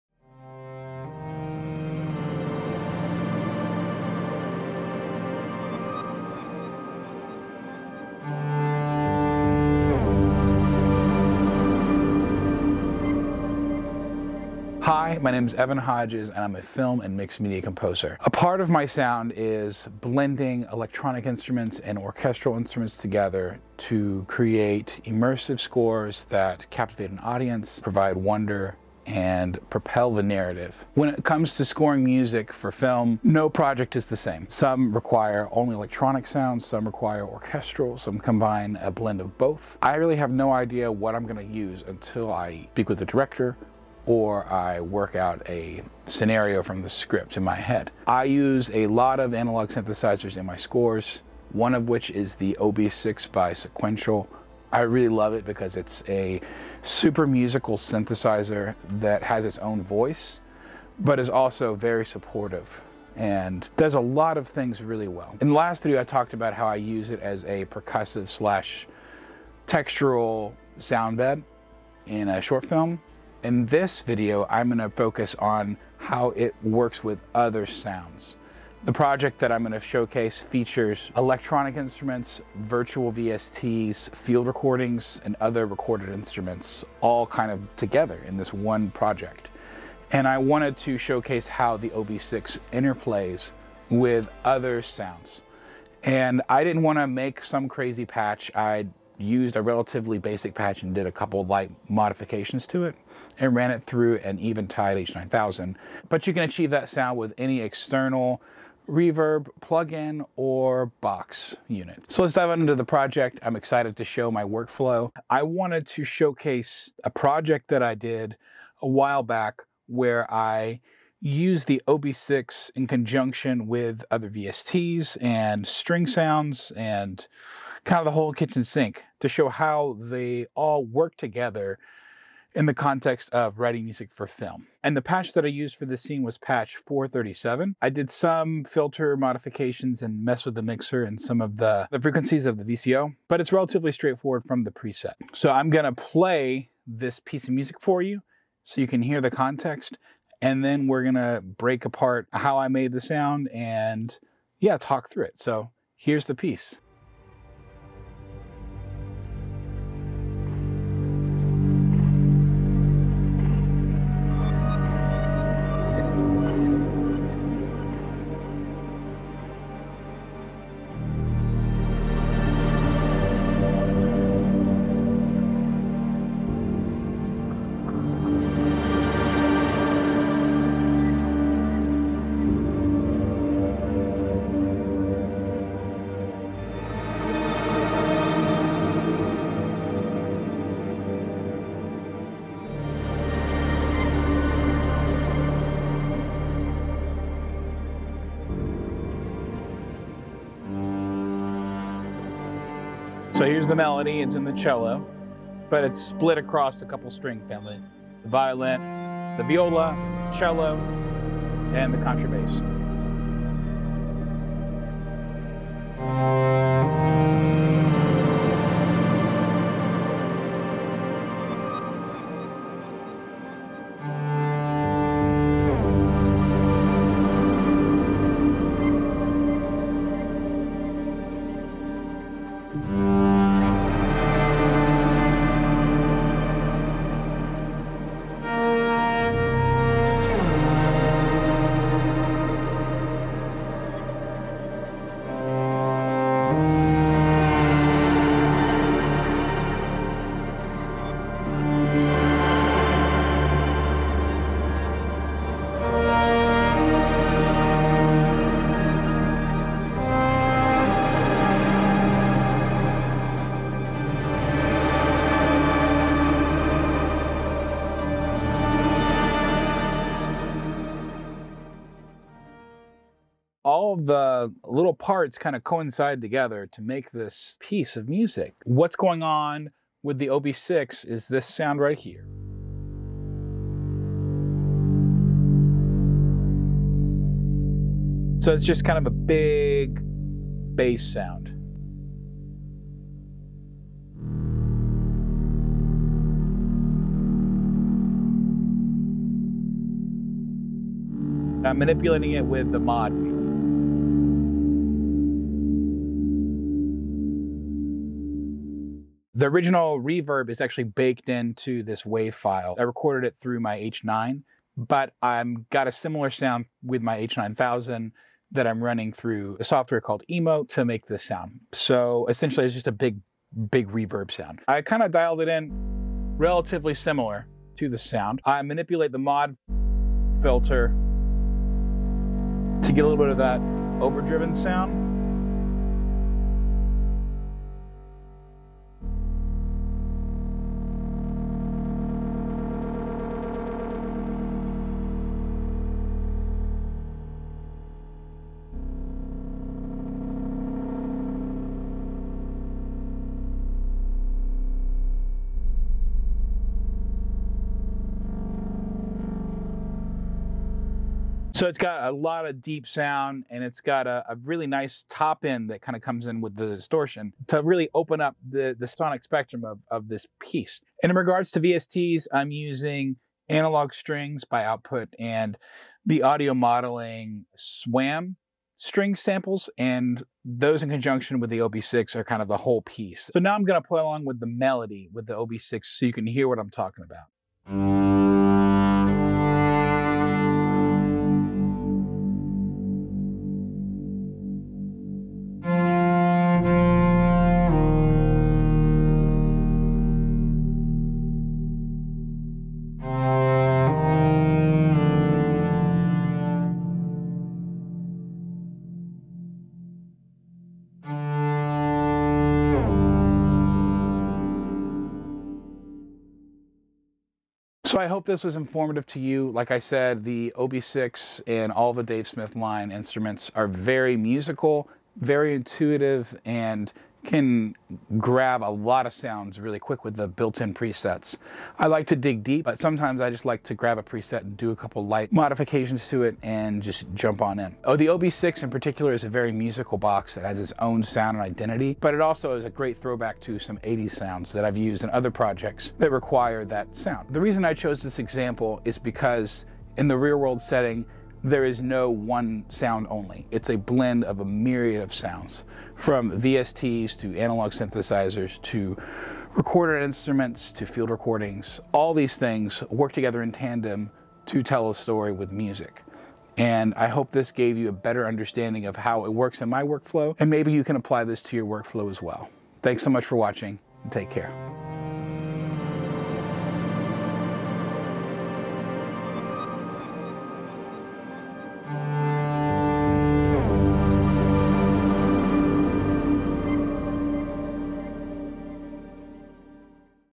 Sequential Synth Tips With Evan Hodges: Scoring For Film With The OB-6, Part 2
Welcome back to “Sequential Synth Tips,” where some of our favorite artists share their tips, tricks, and techniques on a Sequential synth. In this episode, we feature composer Evan Hodges demonstrating the techniques he uses to integrate the OB-6 with other instruments and sounds for a film score.

Evan Hodges is a composer for film and mixed media that holds a degree in Jazz Studies from Georgia State University. He has scored more than 50 films, including both feature films and short films, two musicals, and a full video game soundtrack. Scoring comes intuitively to Hodges. His background and training in jazz, with its highly improvisational component, allows him to adapt quickly and easily to score both simple and advanced thematic musical cues appropriate for every scene. In 2017, Hodges scored the feature, The Canadoo. He was Emmy-nominated in 2018 for the PBS documentary feature, My Dear Children.